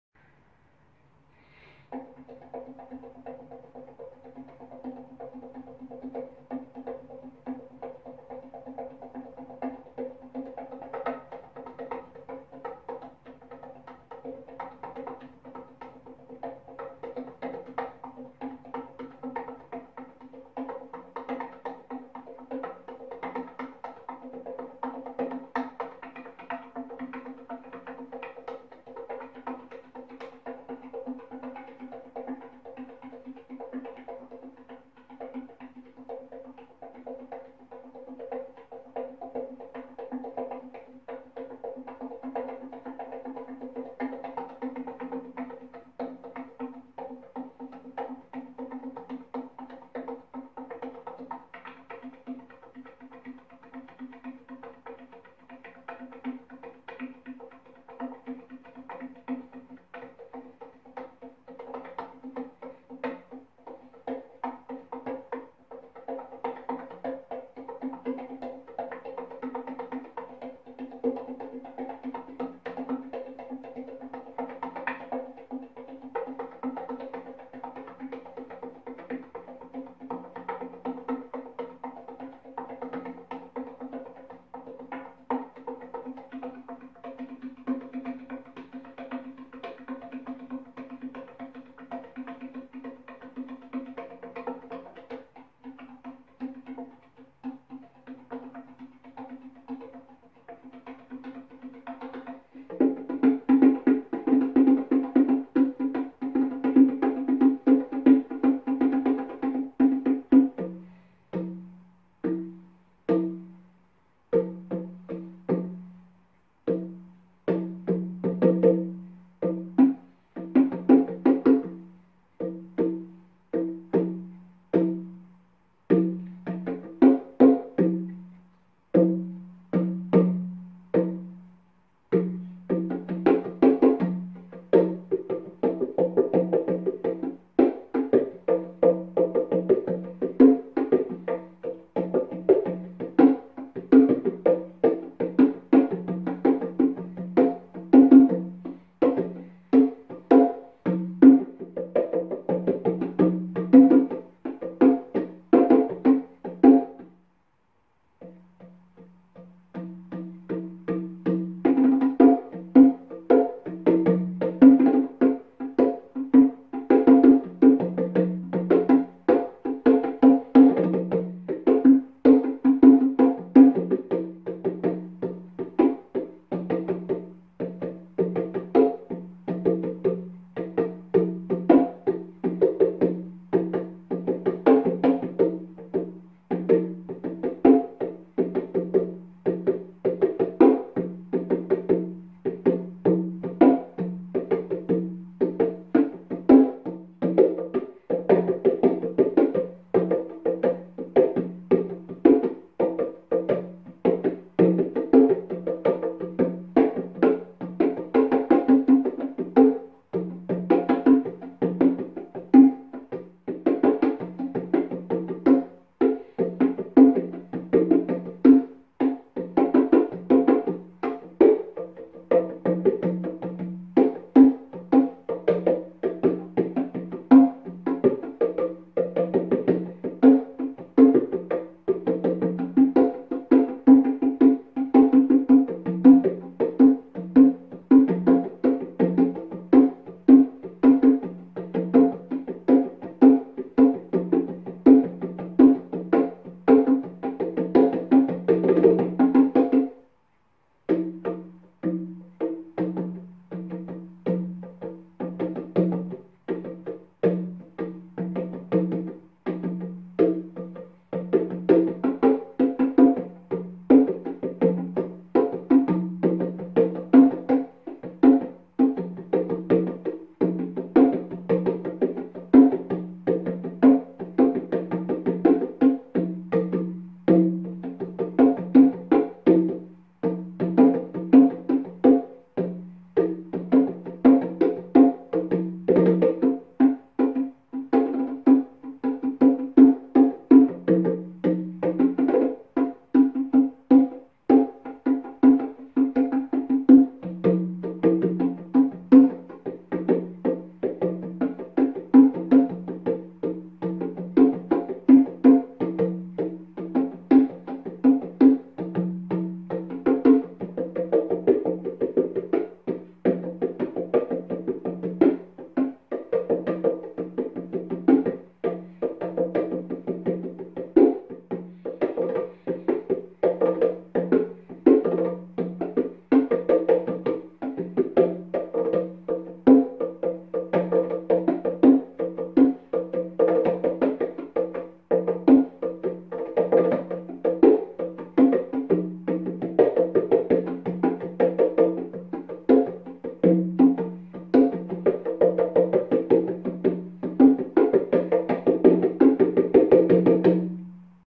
Quarantine Bongo Jam
A video I did back when quarantine started. It might be kind of hard to tell, but I'm playing on the shells of my bongos in the beginning. Sorry about the bald spot. It doesn't go away.